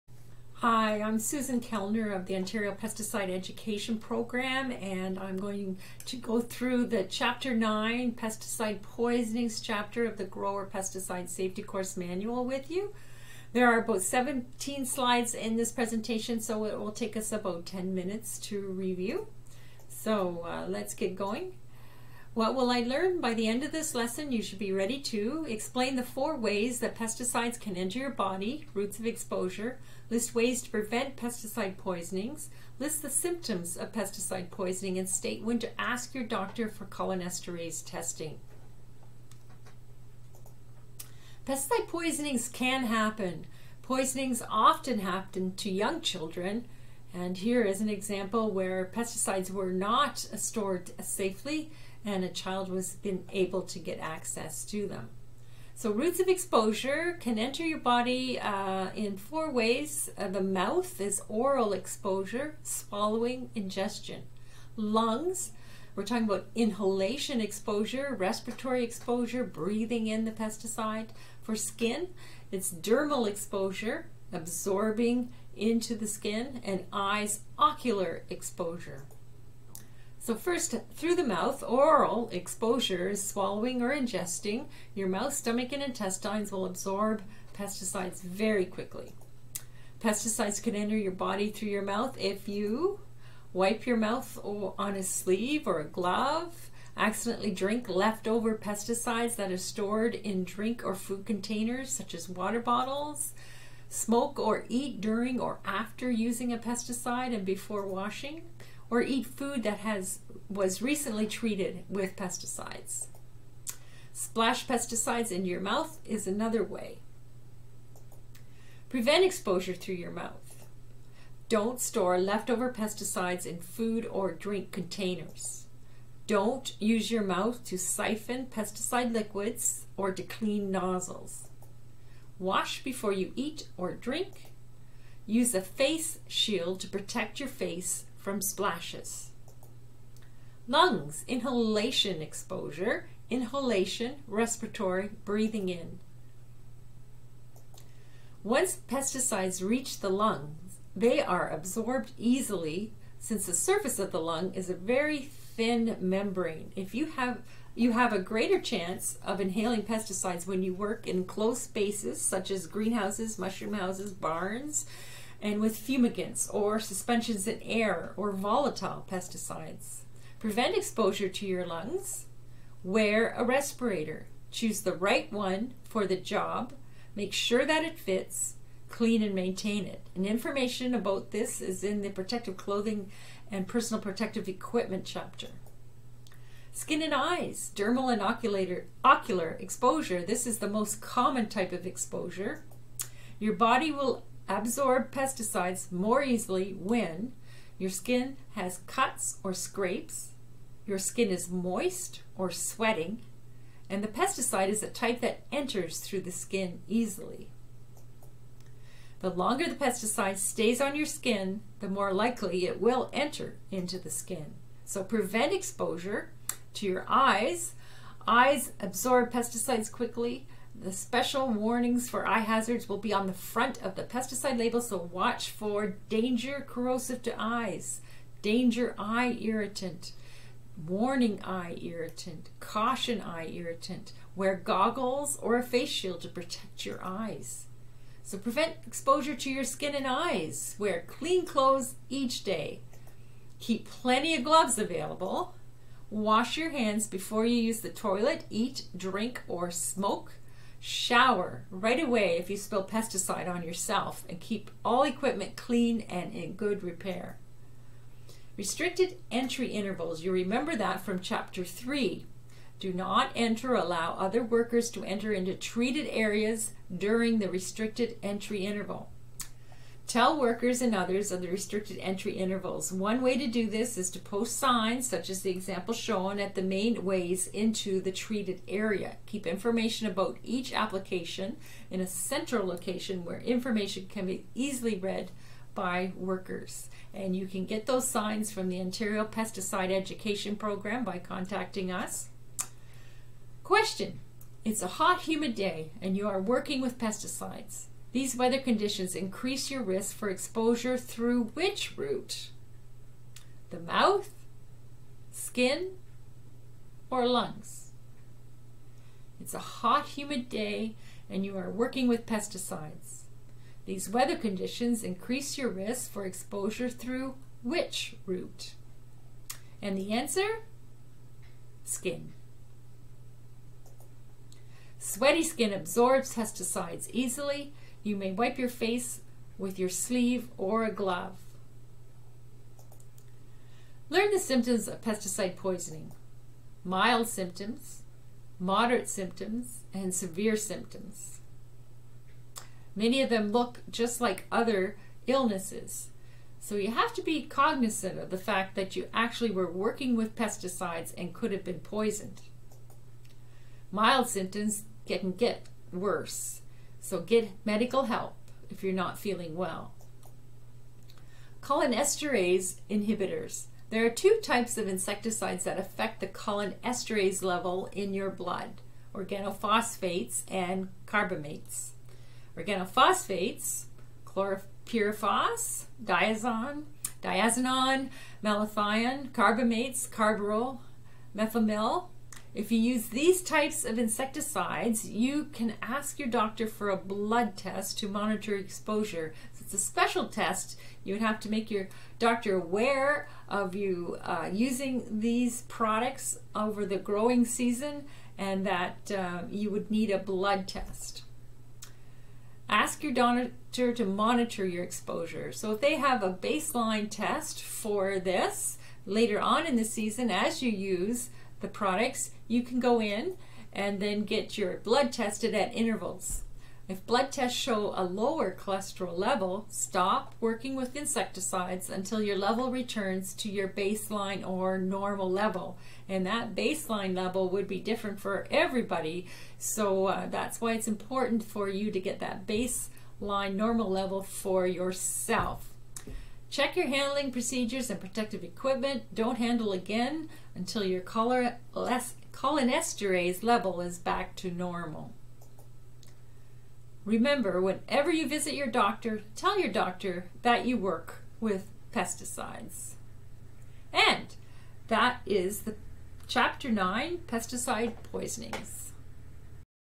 09 Pesticide Poisonings Grower Pestcide Safety Course Manual
A presentation of chapter 9 of the Grower Pesticide Safety Course Manual.  This manual is used for the certification of farmers in Ontario for the purchase and use of Class B, Restricted pesticides and Class C, Agricultural pesticides.